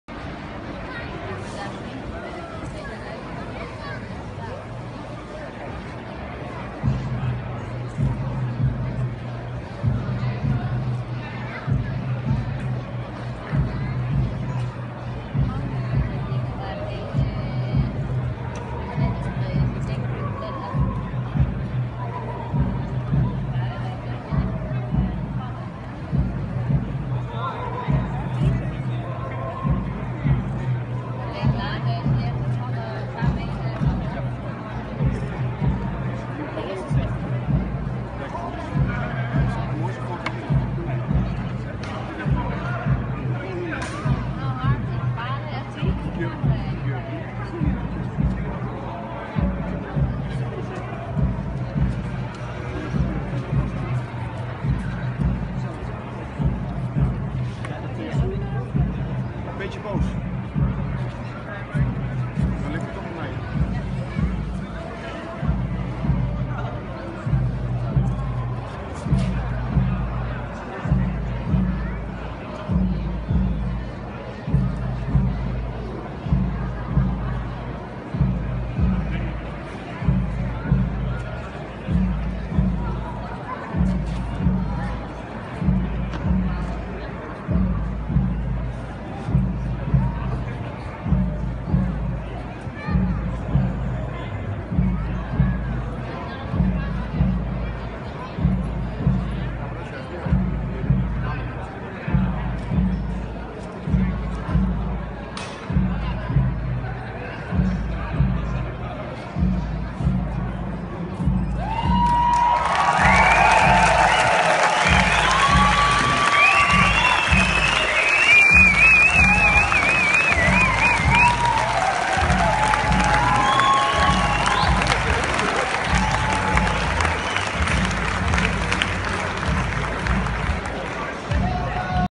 Cirque Pardi! op De Kaaij (2015)
Cirque Pardi! op De Kaaij in Nijmegen op 14, 15, 21 en 22 augustus 2015.